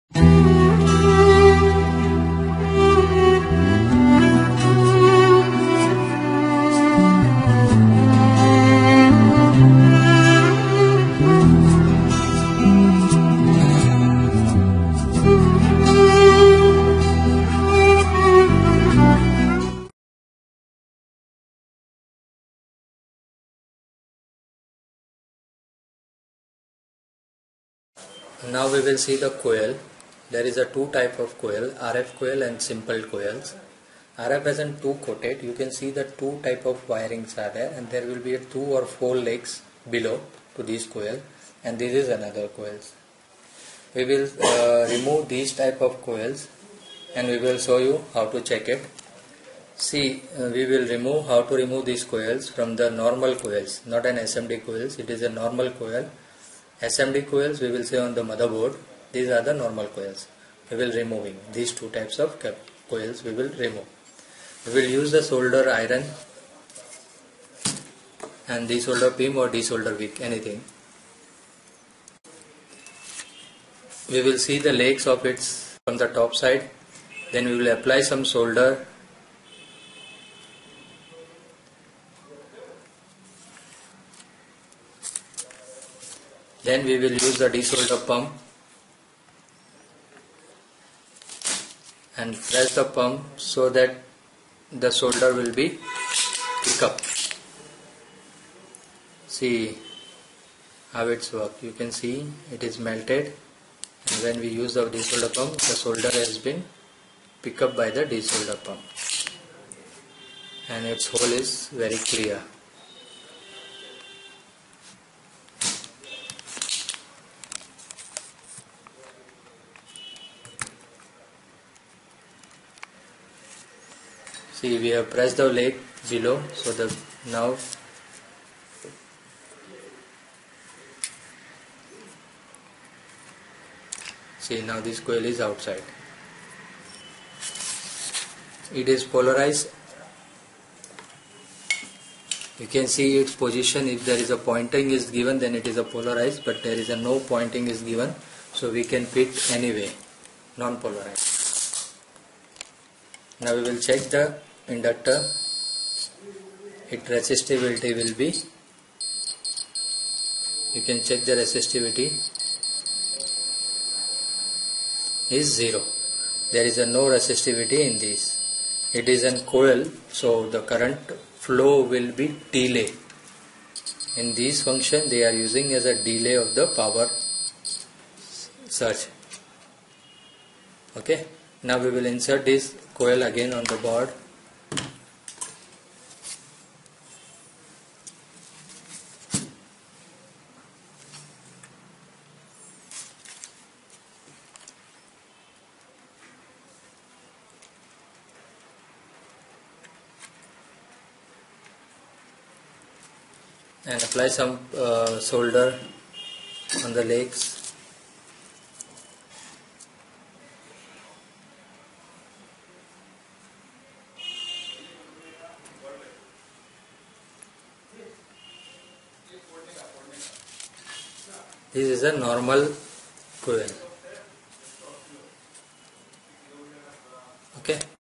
Removing,Testing, Identify Coil/Inductor SMD and Through Hole Component (English)
Intersoft Institute Training Center

Online Training Available over the globe, Online/Offline
IAO | ISO Certified Institute